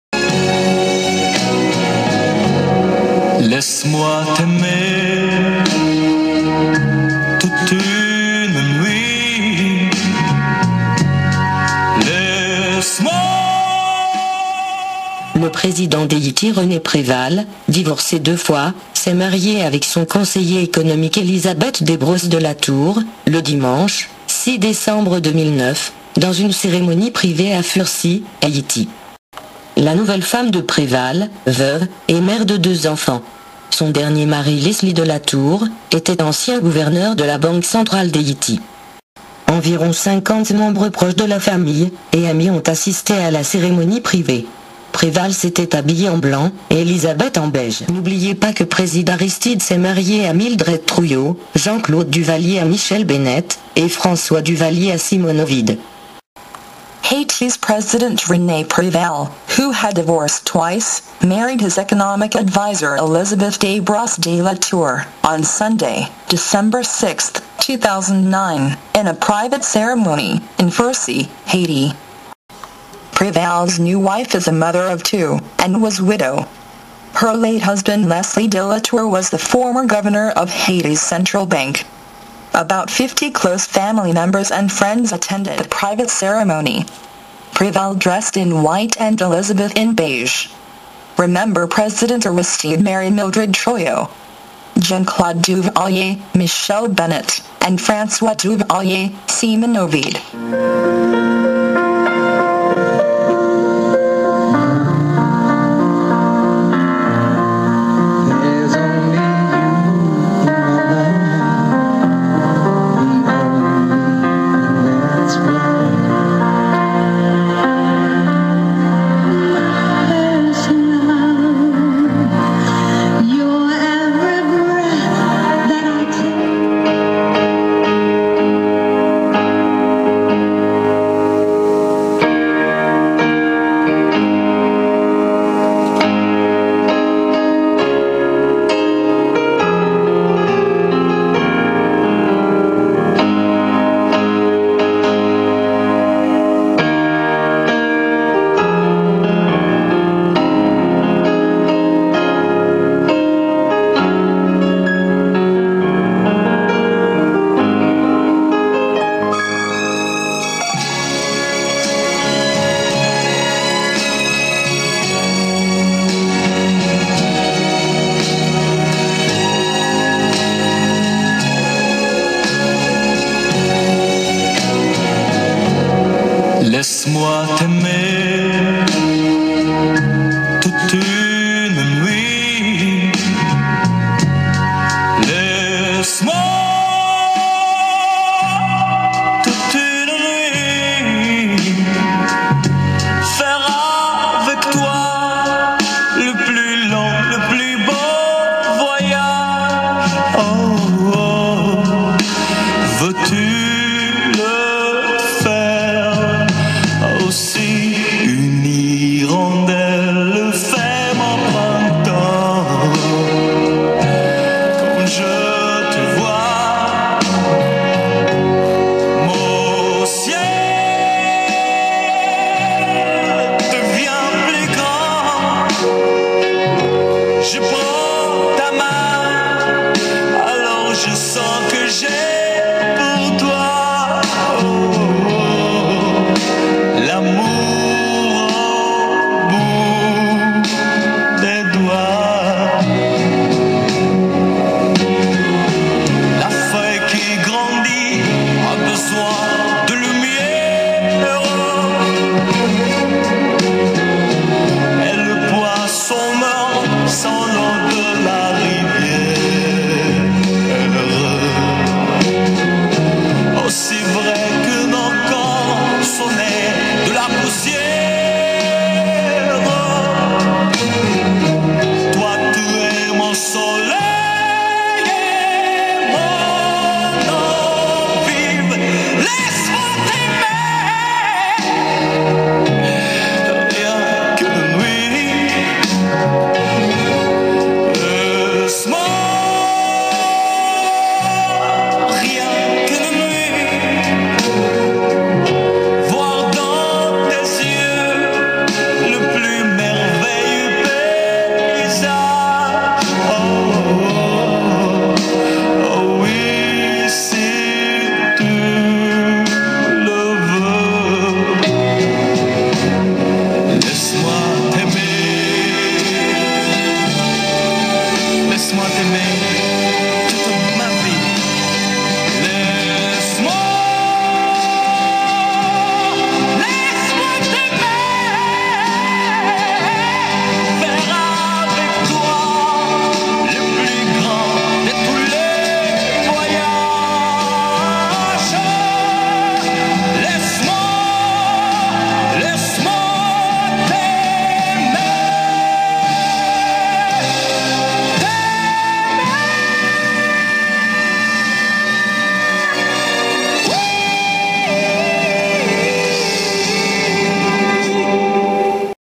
Haiti Pres. Preval Marries Sweetheart Elisabeth Debrosse
President Rene Preval of Haiti Marries his Sweetheart Elisabeth Debrosse.

The 65- year-old president of Haiti Rene Preval married his 47-year-old darling and economic advisor Elisabeth Debrosse  in a private ceremony on Sunday, December 6, 2009 in Haiti.  Elisabeth, a widow and mother of two, was married to Leslie Delatour, the former governor of Haiti's central bank.  About 50 close family members and friends attended the nuptial ceremony.